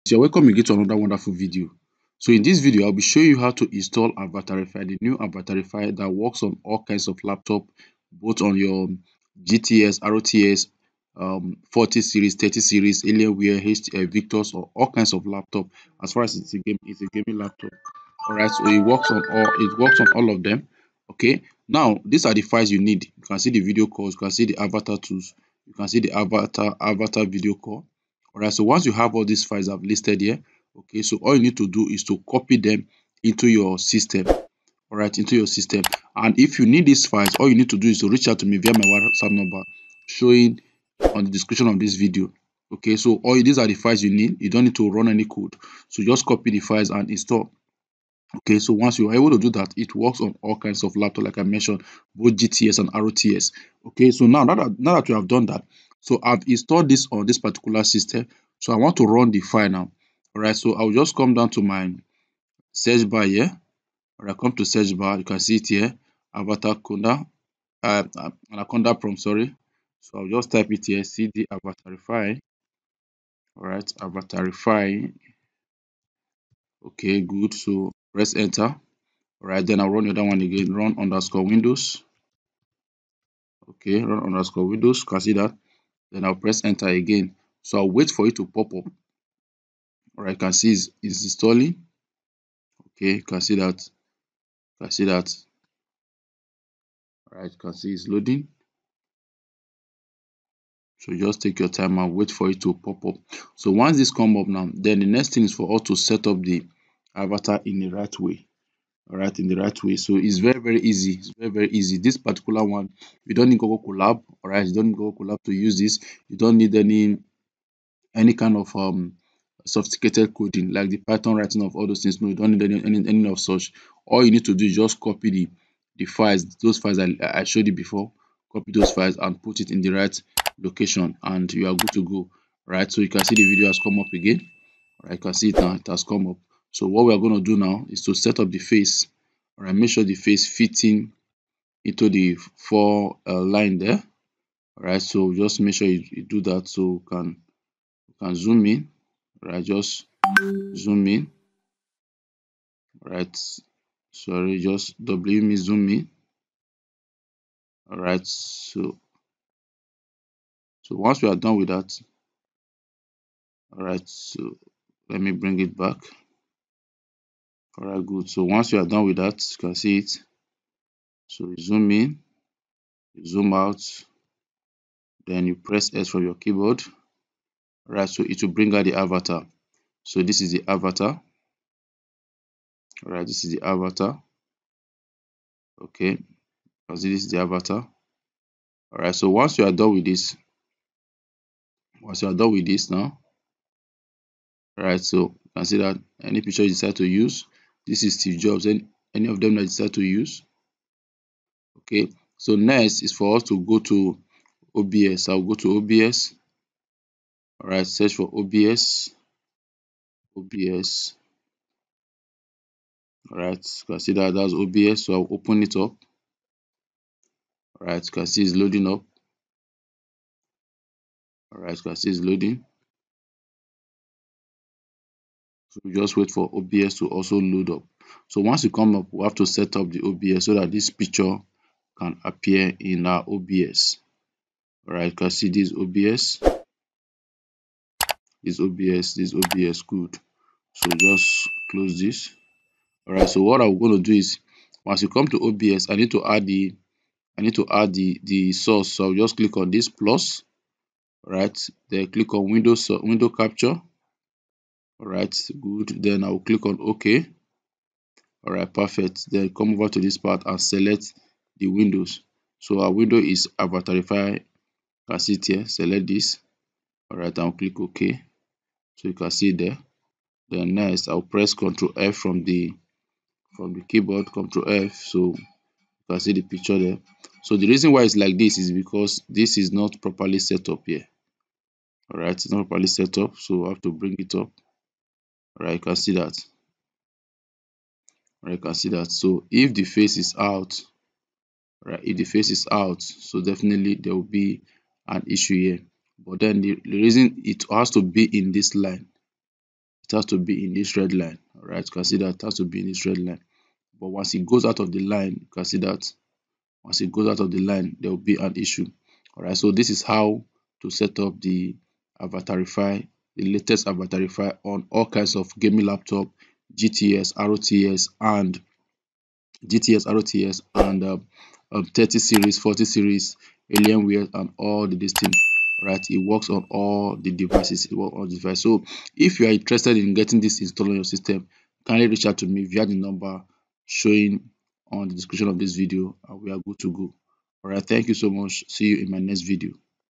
How to Install Avatarify on All Kinds of system RTX and GTX | Step-by-Step Guide
How to Install Avatarify on RTX and GTX Systems | Step-by-Step Guide 🎭

Want to bring your video calls to life with real-time face-swapping? In this video, we’ll guide you through installing Avatarify on any system, whether you’re rocking an RTX or GTX GPU. Avatarify lets you mimic your favorite characters or transform into fun avatars during Zoom, Teams, or any virtual meeting!

🎥 What You'll Learn:
✅ System requirements for RTX and GTX setups
✅ Optimizing performance for smooth results

⚙️ Supported Platforms:

Windows
macOS
Linux
📌 Quick Links in the Video:
0:00 Intro to Avatarify
1:15 Checking System Compatibility
3:00 Download and Installation
6:30 Configuration for RTX and GTX GPUs
9:00 Testing and Final Tips